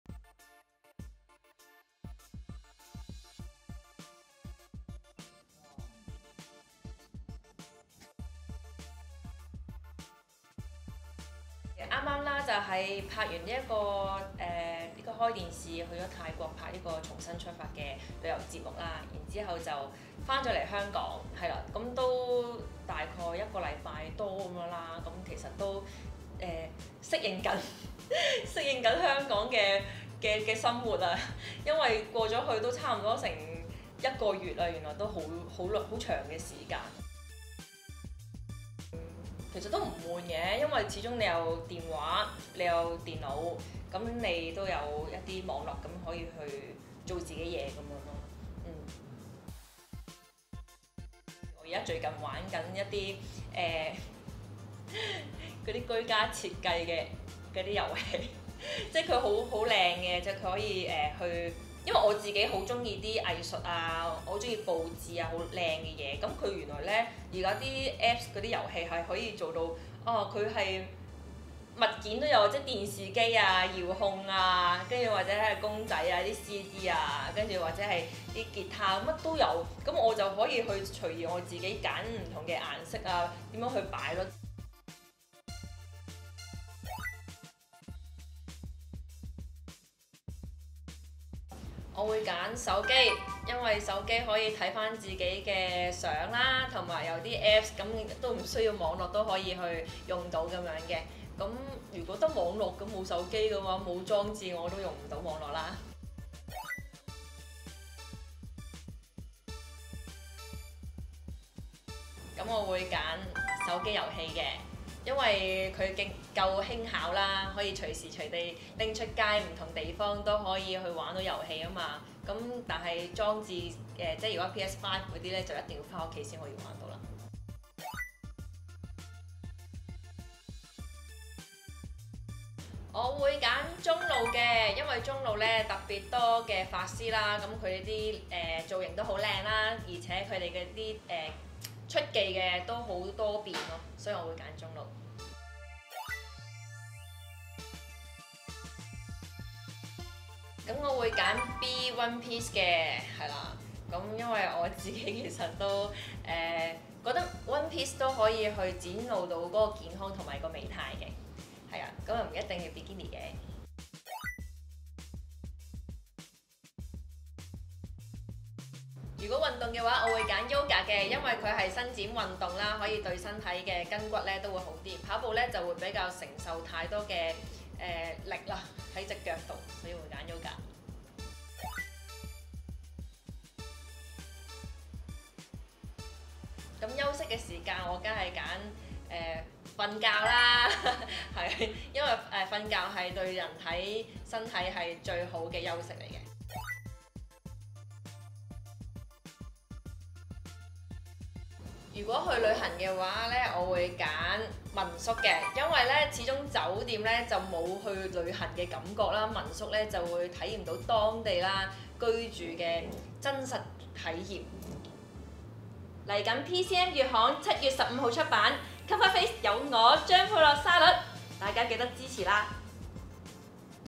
封面人物張沛樂（沙律）快問快答 ︳泰國拍攝開電視節目《重新出發》趣聞 ︳PCM 月刊八月號
全新《PCM》月刊第一期，我們請到剛完成泰國拍攝回來的拍攝的張沛樂（沙律）作為封面人物，除了講講拍攝今次開電視節目《重新出發》的趣事之外，亦都同大家來個快問快答，了解佢更多！

Chapters 章節：
00:00 開始
00:08 最新動向
00:55 快問快答 2 選 1
03:35 預告

封面人物： Charlotte Cheung - 沙律
封面攝影： Szetosifu 司徒夾帶
化妝：Eliza Chan  ︳影片：Pong  ︳製作：Jacky